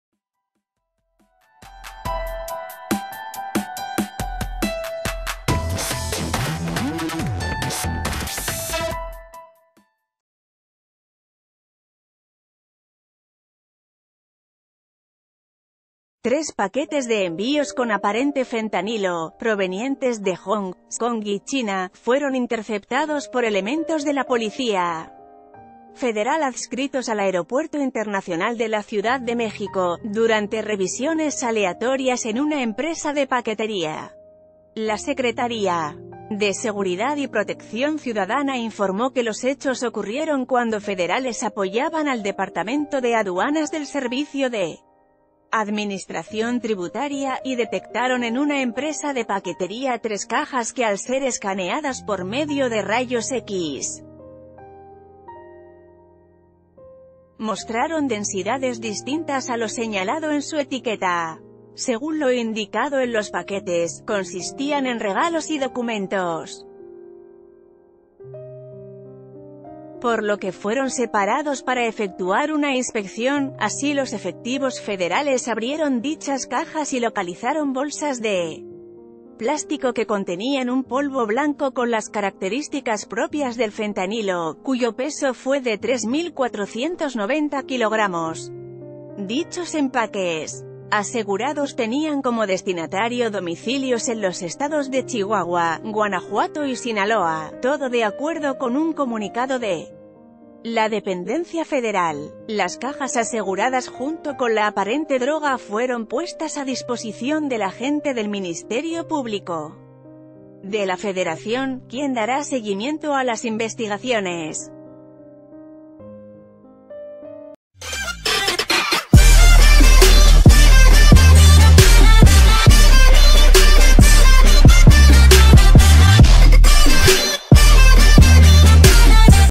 Aseguran cajas con fentanilo de China en el AICM
Tres paquetes de envíos con aparente fentanilo, provenientes de Hong kong y China, fueron interceptados por elementos de la Policía Federal adscritos al Aeropuerto Internacional de la Ciudad de México, durante revisiones aleatorias en una empresa de paquetería. La Secretaría de Seguridad y Protecció...